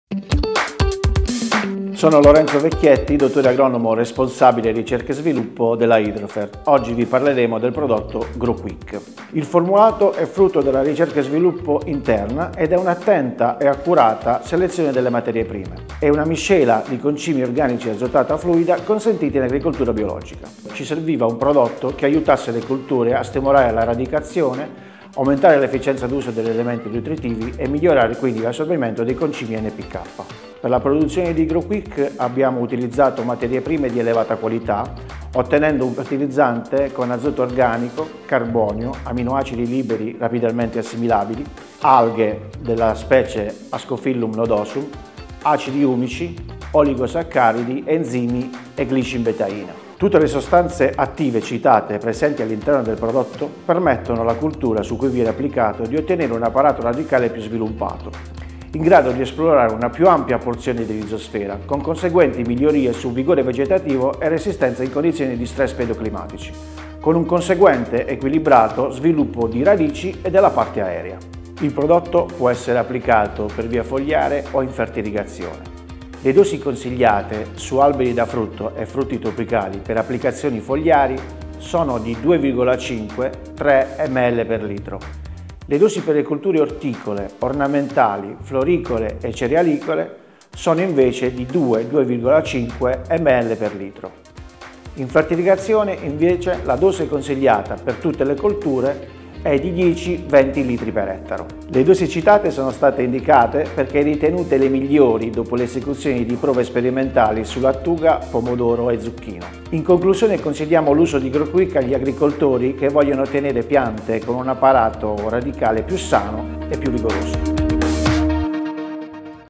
"Focus on" Grow Quick | Hydro Fert (English version)
We introduce our new "Focus on" serie which aims to help customers and not yet customers to learn more about our products. In each video we will focus on the following key points:
- what is the product?
- why did we conceive it?
- what does it contain?
- what are its results?
- how is it applied?
in order to suggest to farmers all the necessary elements to better understand if the product is what they are looking for.
We start the serie talking about Grow Quick, a formulation with high technological content, that our Dr. Lorenzo Vecchietti will explain in detail.